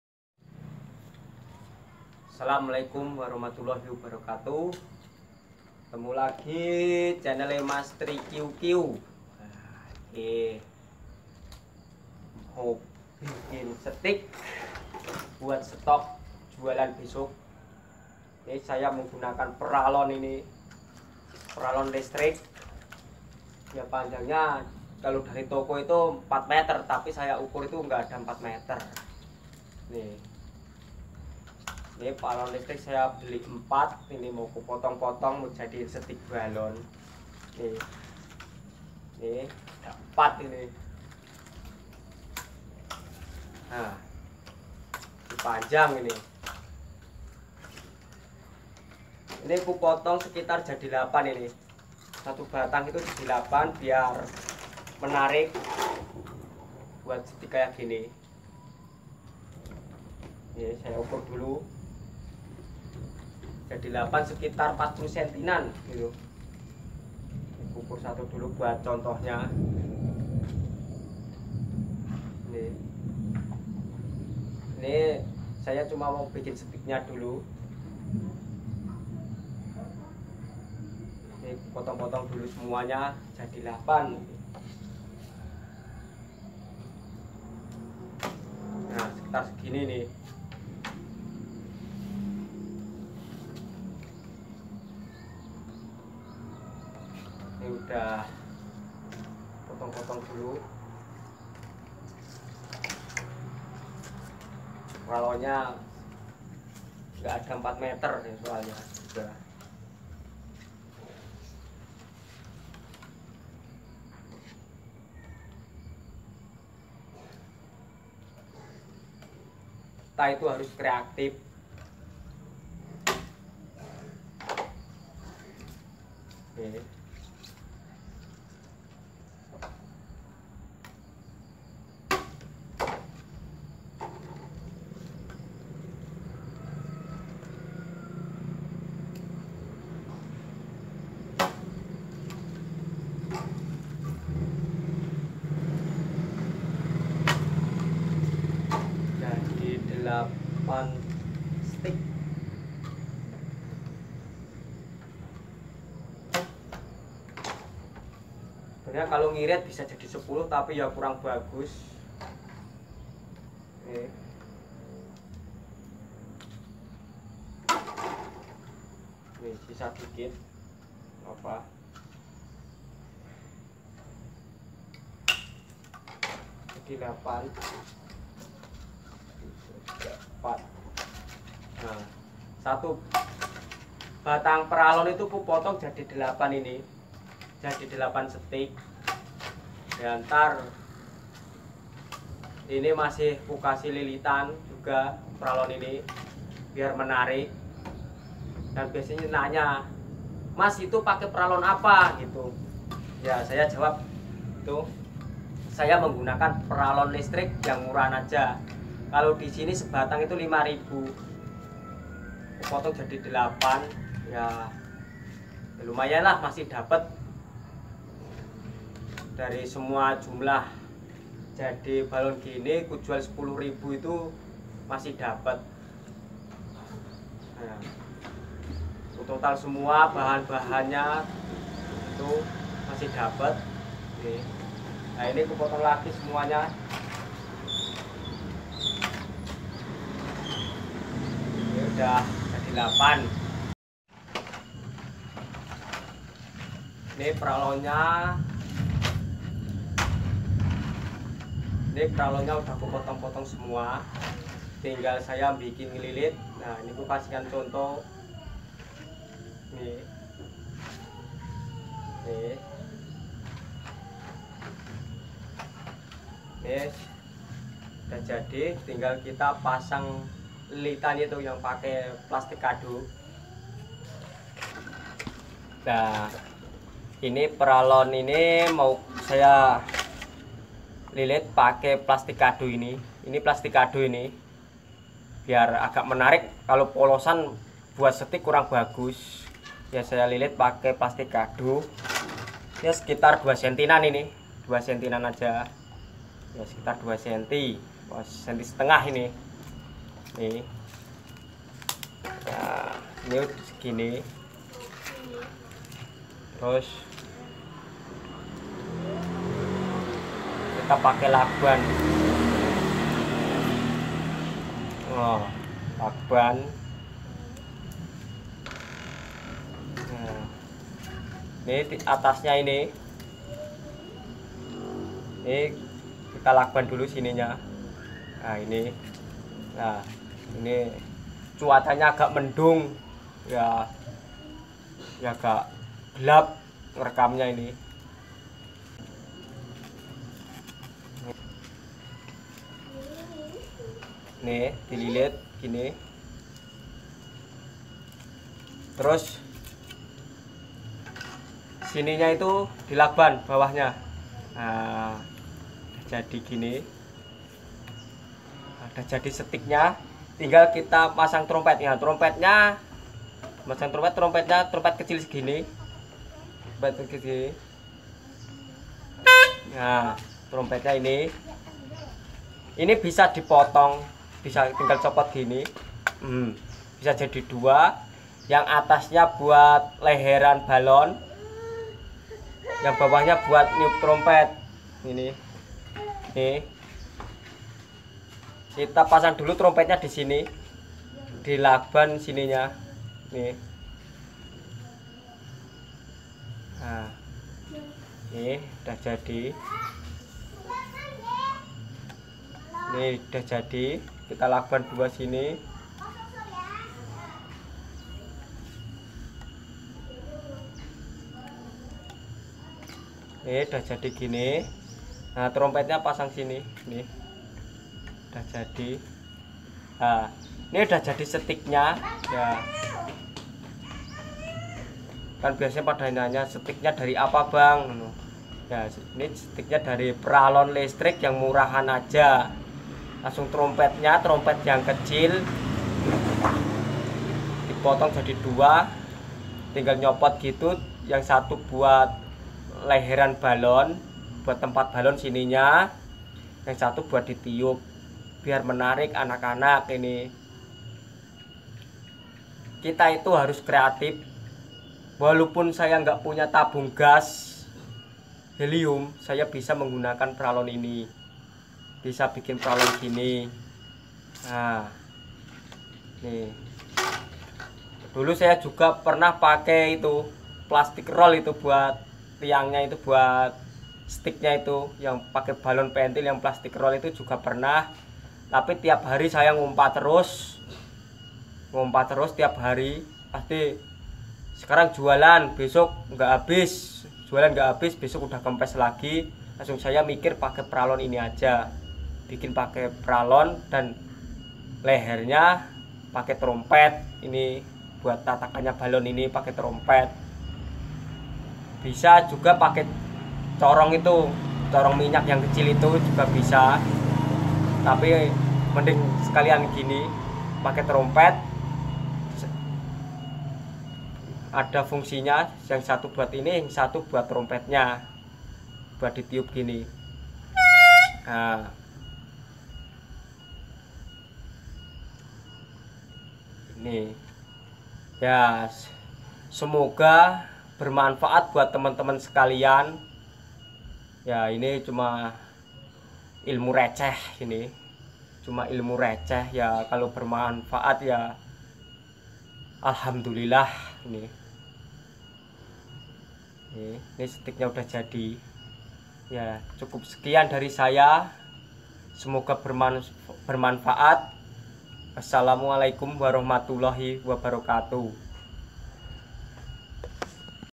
bikin stok stik balon karakter
membuat setik balon karakter menggunakan pralon listrik.